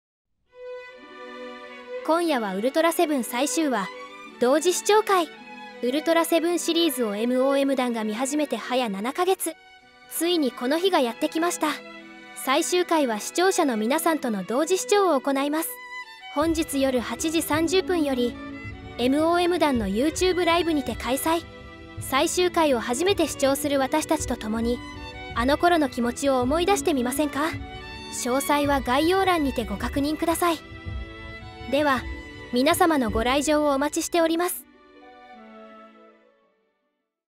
【こんや20時30分】ウルトラセブン最終回「同時視聴」開催のお知らせ【史上最大の侵略】
【ウルトラセブン最終回を一緒に見よう♪】

◆ウルトラセブン第48話・第49話
「史上最大の侵略  前編・後編」
MOM団メンバーとの同時視聴会📡⚡️

◆日付
2023年4月7日(金)

◆時間
20時30分〜（オープニングトーク、本編視聴、視聴後の感想）

◆配信会場
MOM団YouTubeチャンネル内にて
(※放送についての緊急の連絡などはTwitterにて行いますので、MOM団Twitterアカウント のフォローをおすすめしております。

🌸同時視聴の楽しみ方🌸

本編視聴中は【メンバーのリアクション】と、【進行タイムコード】を表示します。
メンバーの本編視聴に合わせて、各自DVDや「円谷イマジネーション」内等で同時にご視聴いただくとより今回の配信が楽しめるかと思います♪
（※ウルトラセブン本編映像や音声を、配信内で使用することは一切ございませんのでご注意ください。）

✅注意点✅

・今回は企画の趣旨上、皆様からのコメントの取り上げが少なくなってしまう場合もございますのでご了承ください。

・視聴前、視聴中のネタバレコメントはお控えください。

・他参加者様への過度な暴言、中傷などがあった場合、強制退出させていただきます。

・急遽開始時間や日程が変更になる場合もございますので、ご了承ください。